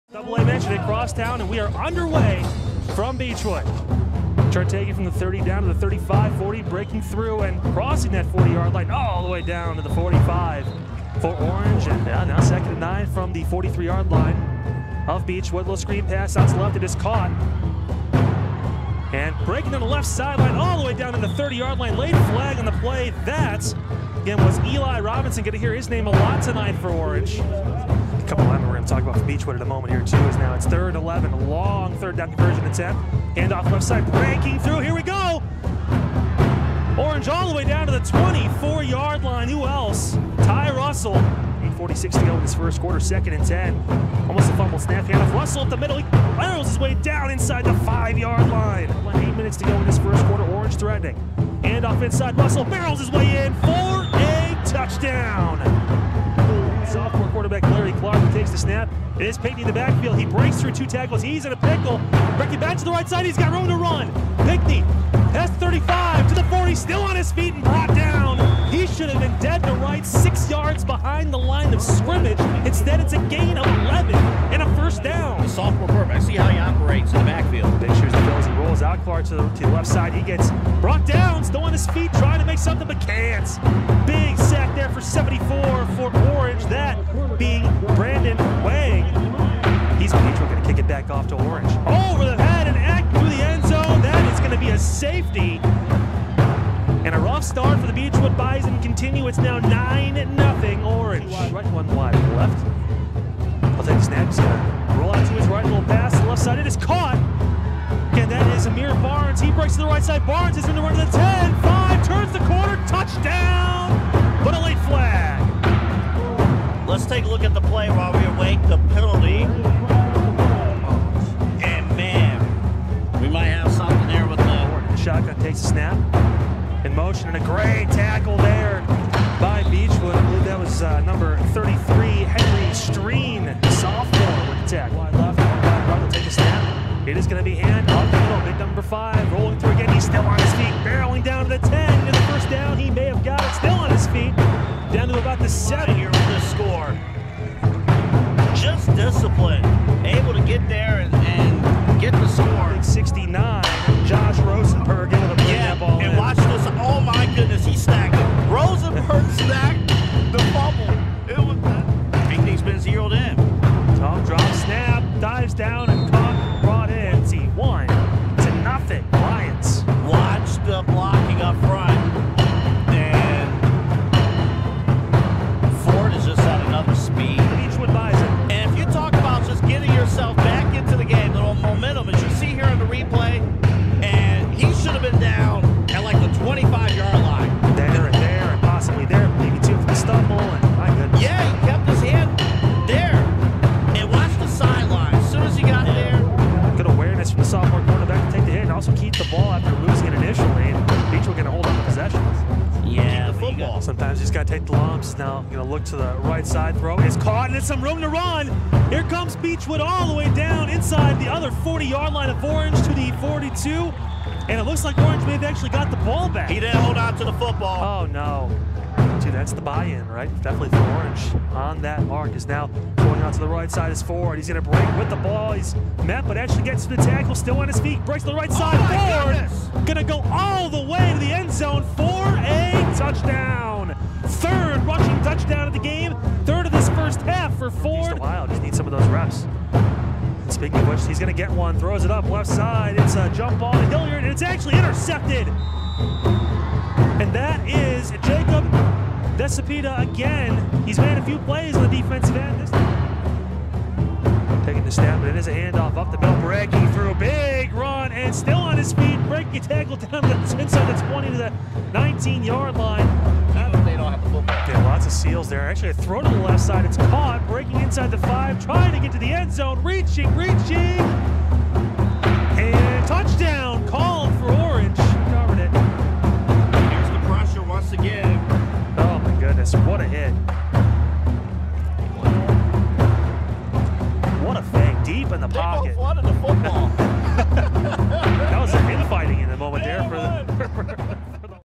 Highlights: Orange knocks off Beachwood
Highlights from Orange's 42-0 season-opening win over Beachwood on Aug. 19, 2023.